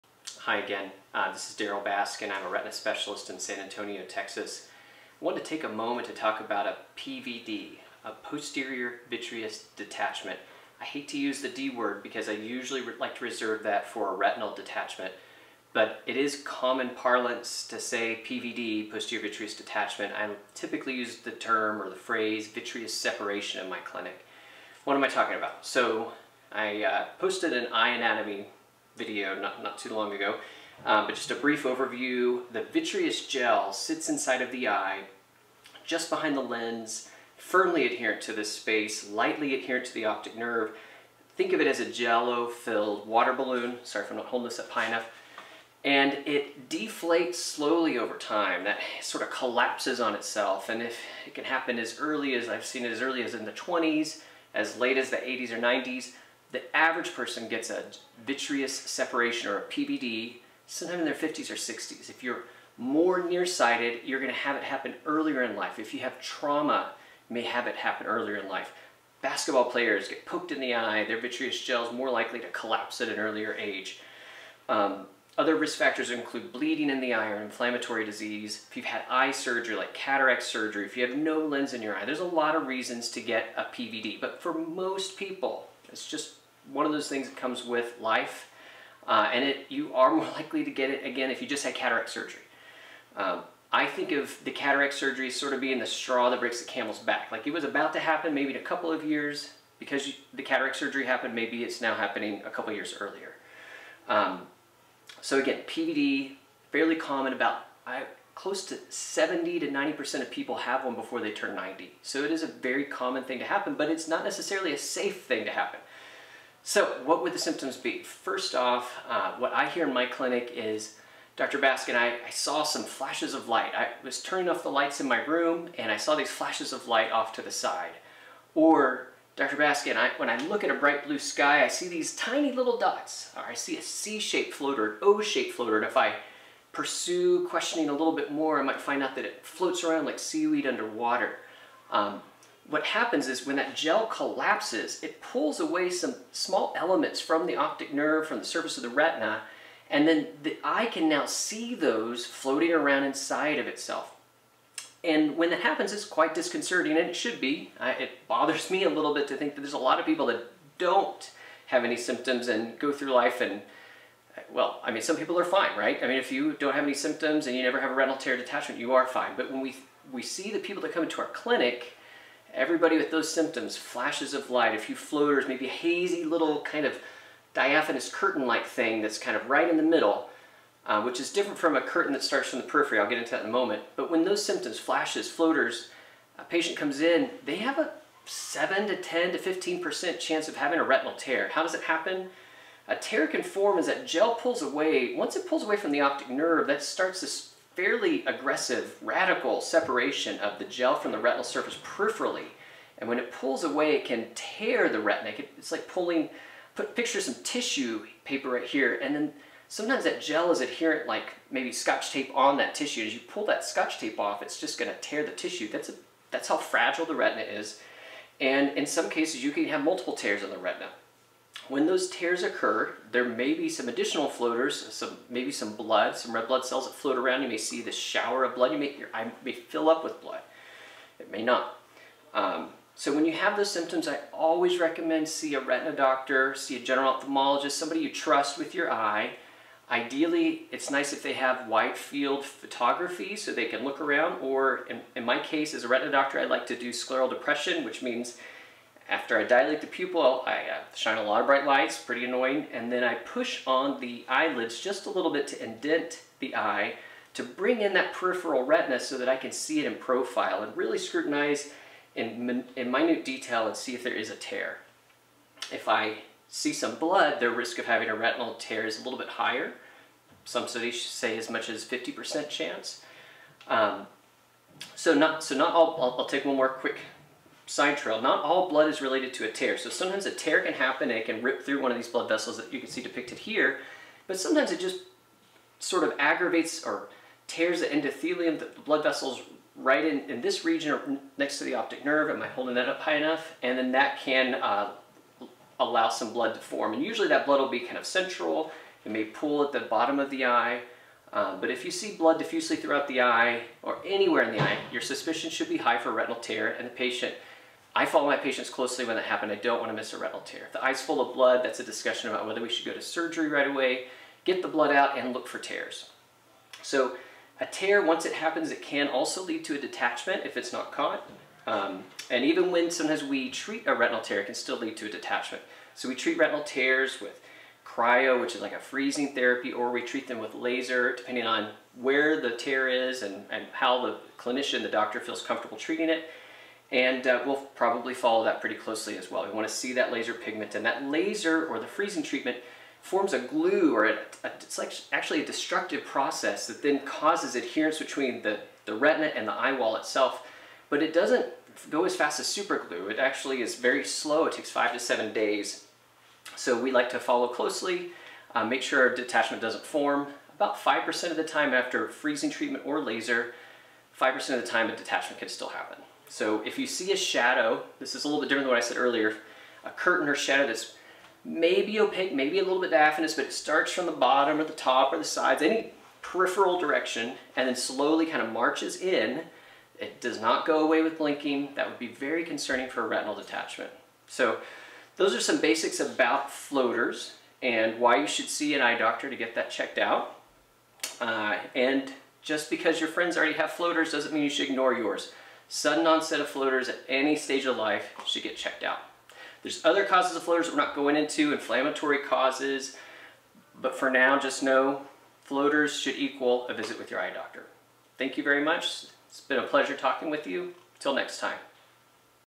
Sudden Onset Floaters For Patients
This is a brief introduction to new-onset floaters for lay people interested in learning about posterior vitreous detachment.